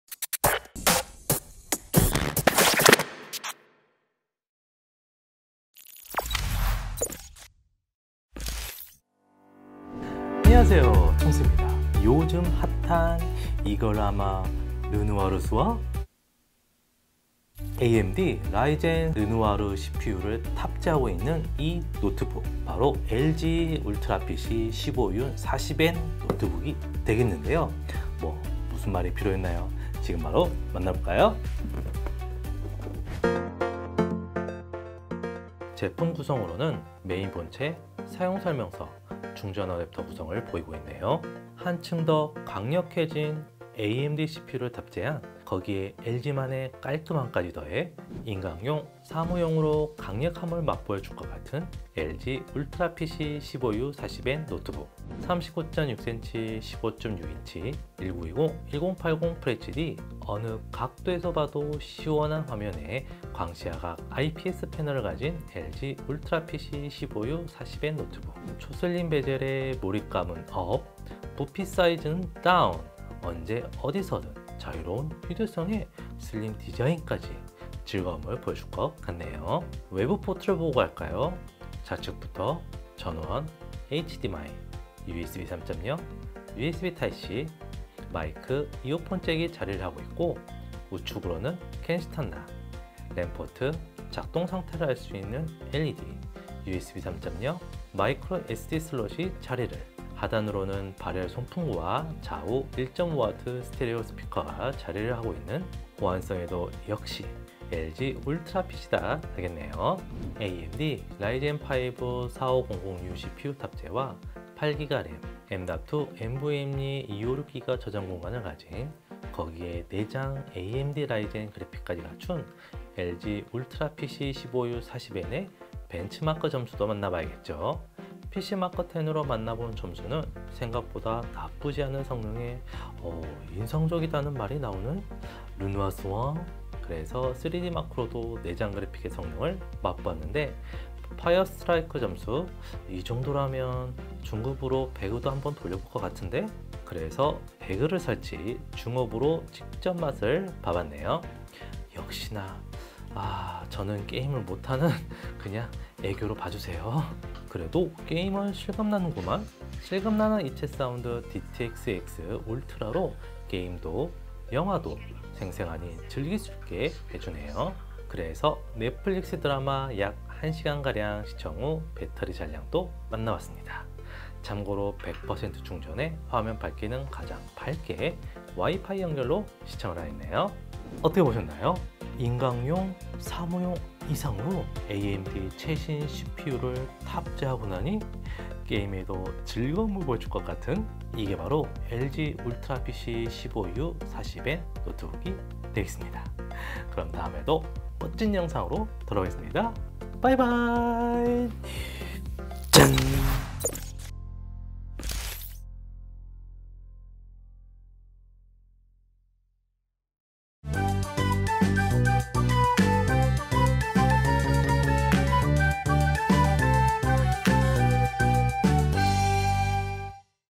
깔끔한 인강용 노트북이 필요해? / LG전자 2020 울트라PC 15U40N-GR56K 리뷰 [노리다]
인강도 듣고 가볍게 게임도 즐겨봐요!
2020년 9월 표준 노트북 선정제품입니다.
다나와에서 무료 대여/제작비 지원

Presented by 비교하고 잘 사는, 다나와